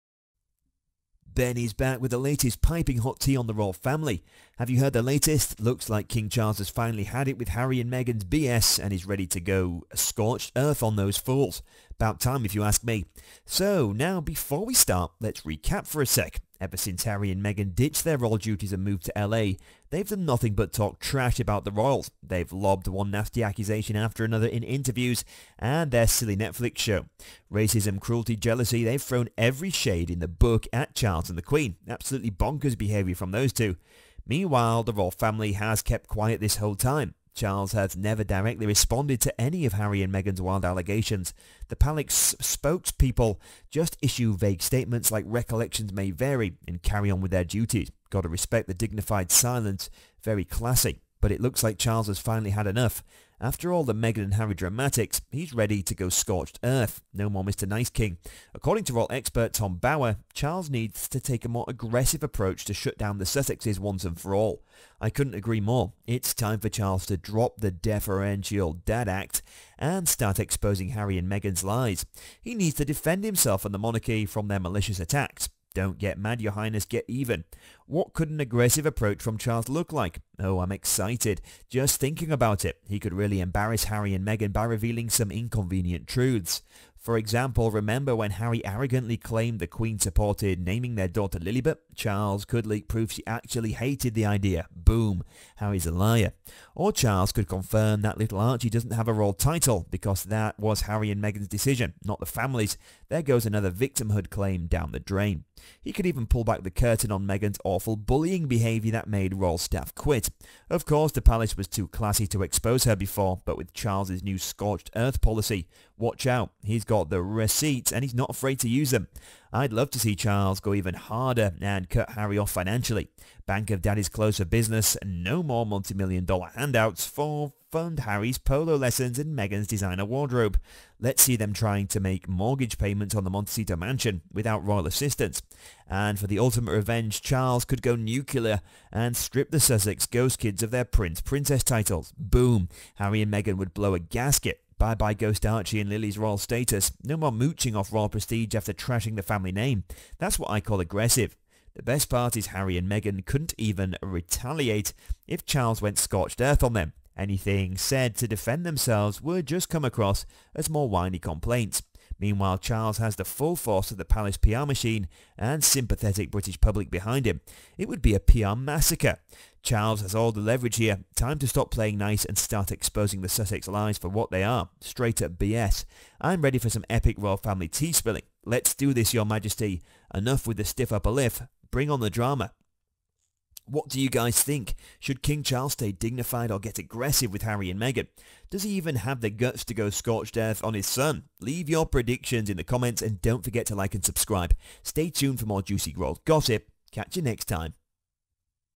KING CHARLES GOES NUCLEAR ON MEGHAN! Meghan In Tears BEGS For King Forgiveness: YOU ASKED FOR IT MEG
Hey everyone! Everyone's favorite, Ben is back with the juiciest royal family updates! King Charles is reportedly fed up with Harry and Meghan's antics and is gearing up for a no-holds-barred response. In this video, we break down the potential scorched earth tactics Charles could use to expose the Sussexes' lies. From leaking inconvenient truths to cutting off financial support, this could be the royal family showdown we've all been waiting for! What are your thoughts on Charles going aggressive? ☕👀